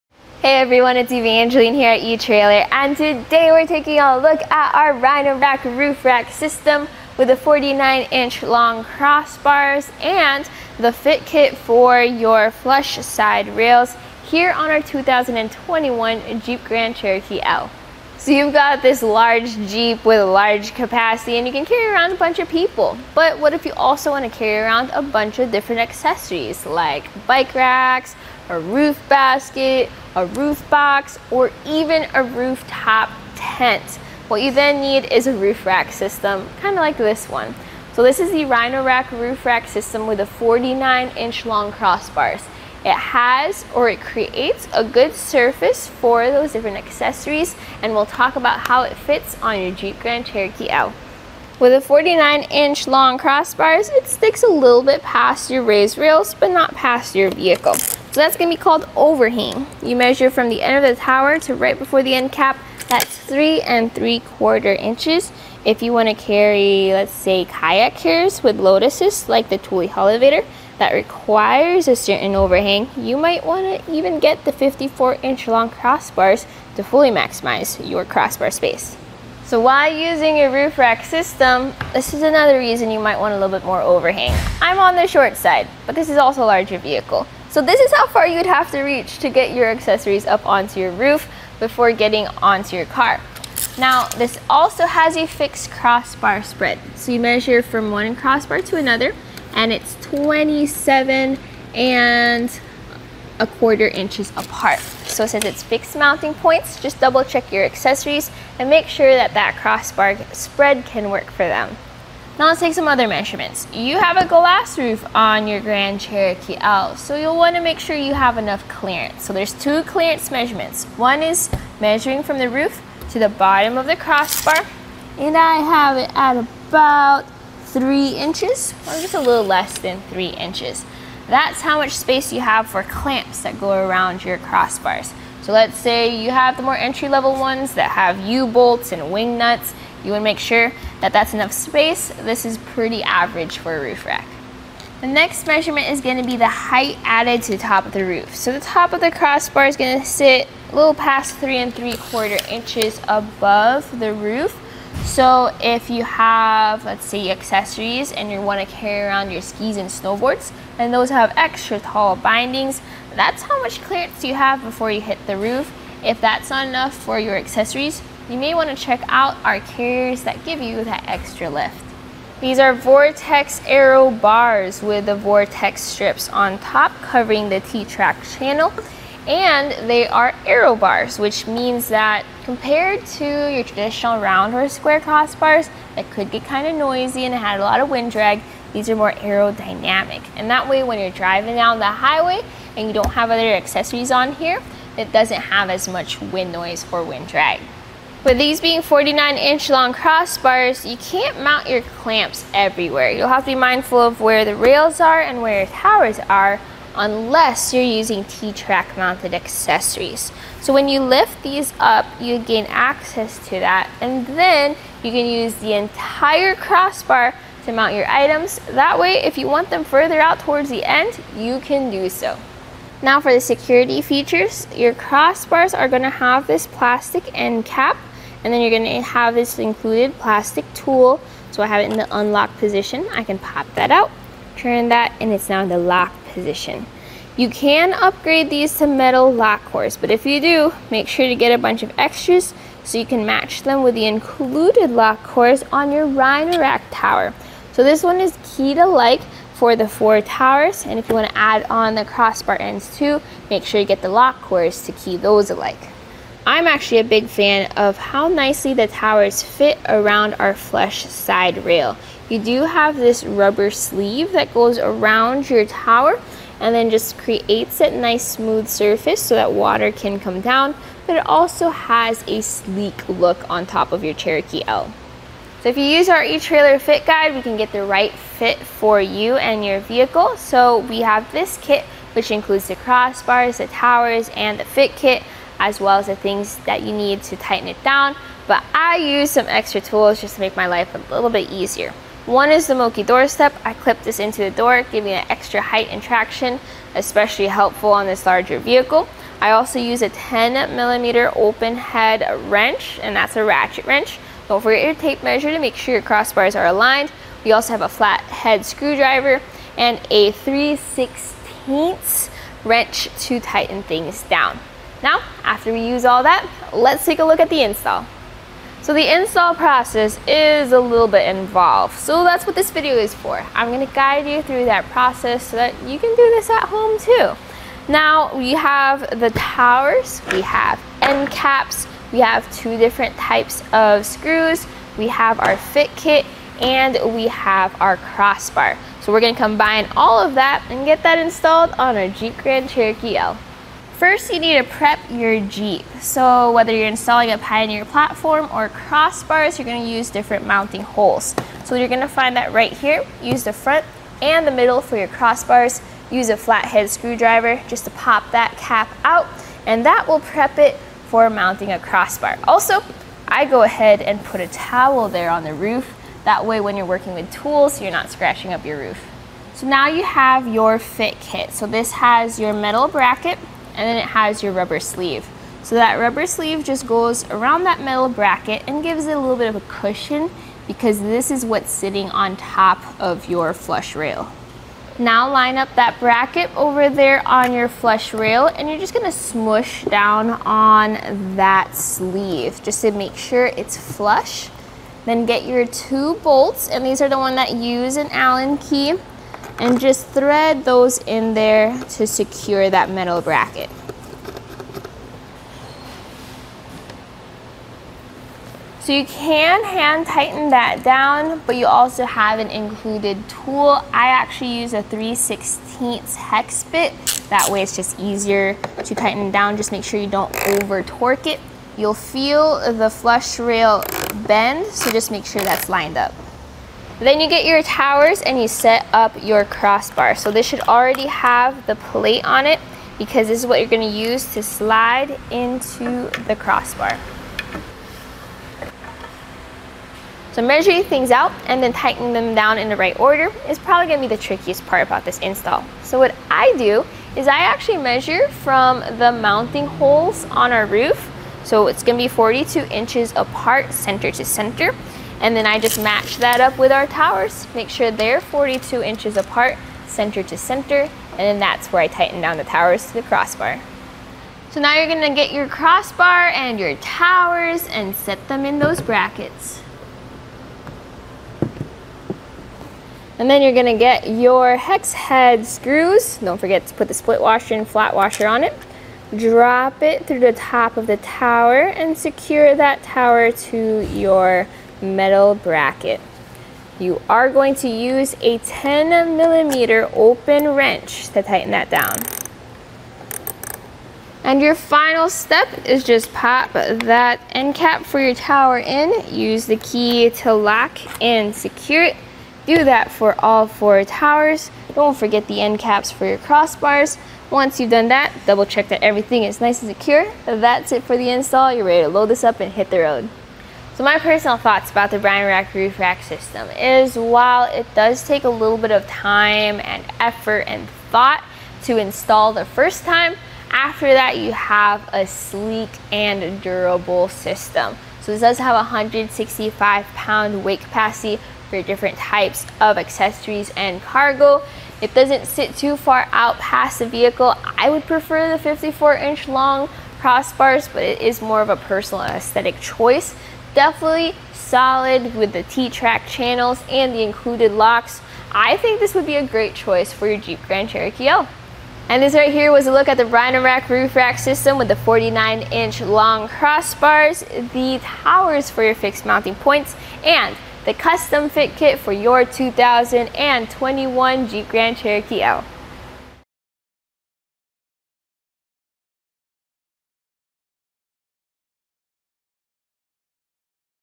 etrailer | Installation Guide for the Rhino-Rack Vortex Roof Rack on a 2021 Jeep Grand Cherokee L
Hey everyone, it's Evangeline here at etrailer. And today we're taking a look at our Rhino-Rack roof rack system for the 49 inch long crossbars and the fit kit for your flush side rails here on our 2021 Jeep Grand Cherokee L. So you've got this large Jeep with a large capacity and you can carry around a bunch of people. But what if you also wanna carry around a bunch of different accessories, like bike racks, a roof basket, a roof box, or even a rooftop tent. What you then need is a roof rack system kind of like this one. So this is the Rhino-Rack roof rack system with the 49 inch long crossbars.

It has, or it creates a good surface for those different accessories. And we'll talk about how it fits on your Jeep Grand Cherokee L. With the 49 inch long crossbars, it sticks a little bit past your raised rails but not past your vehicle. So that's gonna be called overhang. You measure from the end of the tower to right before the end cap, that's 3 and 3 3/4 inches.

If you wanna carry, let's say kayak carriers with load assists like the Thule Hullavator that requires a certain overhang, you might wanna even get the 54 inch long crossbars to fully maximize your crossbar space. So why using your roof rack system This is another reason you might want a little bit more overhang. I'm on the short side but this is also a larger vehicle. So this is how far you would have to reach to get your accessories up onto your roof before getting onto your car. Now, this also has a fixed crossbar spread.

So you measure from one crossbar to another and it's 27 and 3/4 inches apart. So since it's fixed mounting points, just double check your accessories and make sure that that crossbar spread can work for them. Now let's take some other measurements. You have a glass roof on your Grand Cherokee L so you'll wanna make sure you have enough clearance. So there's two clearance measurements.

One is measuring from the roof to the bottom of the crossbar, and I have it at about three inches or just a little less than three inches. That's how much space you have for clamps that go around your crossbars. So let's say you have the more entry level ones that have U-bolts and wing nuts. You wanna make sure that that's enough space. This is pretty average for a roof rack. The next measurement is gonna be the height added to top of the roof. So the top of the crossbar is gonna sit a little past 3 and 3 3/4 inches above the roof. So if you have, let's say accessories and you wanna carry around your skis and snowboards and those have extra tall bindings. That's how much clearance you have before you hit the roof. If that's not enough for your accessories, you may wanna check out our carriers that give you that extra lift. These are vortex aero bars with the vortex strips on top covering the T-track channel. And they are aero bars, which means that compared to your traditional round or square crossbars, it could be kind of noisy and it had a lot of wind drag. These are more aerodynamic. And that way, when you're driving down the highway and you don't have other accessories on here, it doesn't have as much wind noise for wind drag. But these being 49 inch long crossbars, you can't mount your clamps everywhere. You'll have to be mindful of where the rails are and where your towers are unless you're using T-track mounted accessories. So when you lift these up, you gain access to that and then you can use the entire crossbar to mount your items, that way, if you want them further out towards the end, you can do so. Now for the security features. Your crossba